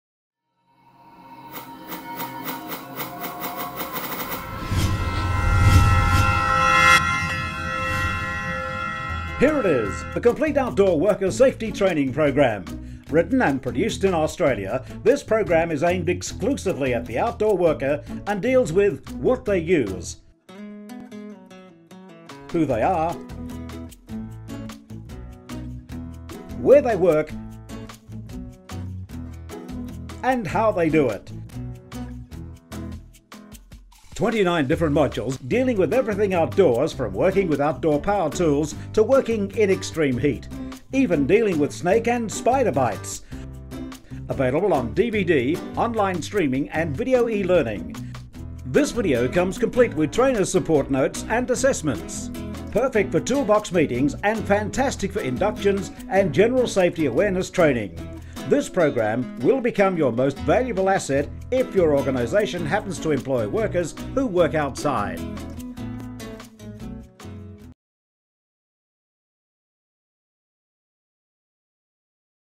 Outdoor Worker Safety - Training Video Trailer
This Outdoor Worker Safety Training Video Series is aimed exclusively at the outdoor worker and covers 29 different safety topics.